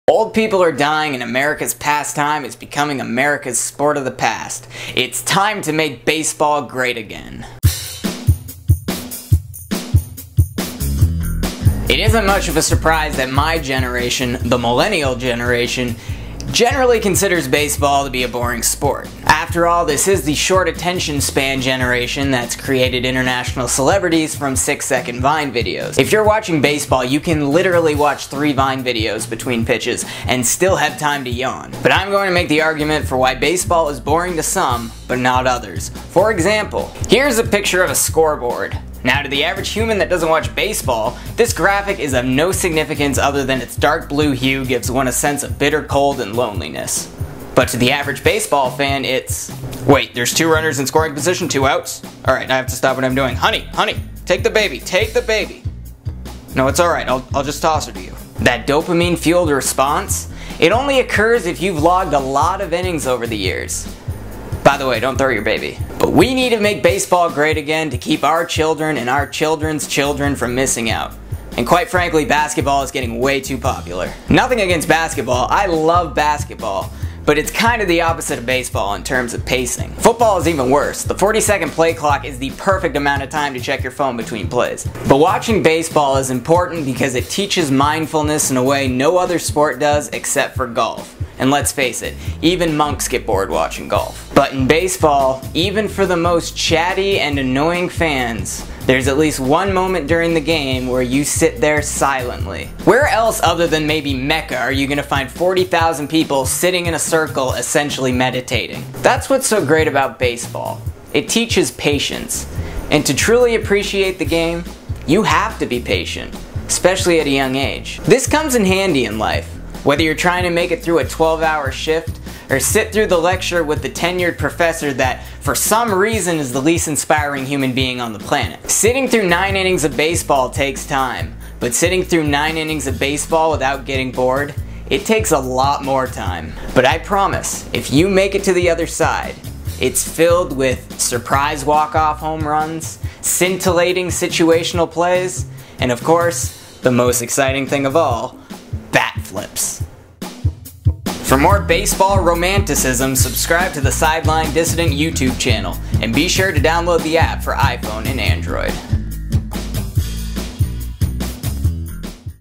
Why We Need Baseball Now More Than Ever
Why watching baseball is a practice of mindfulness, which is critical in today’s short-attention span world.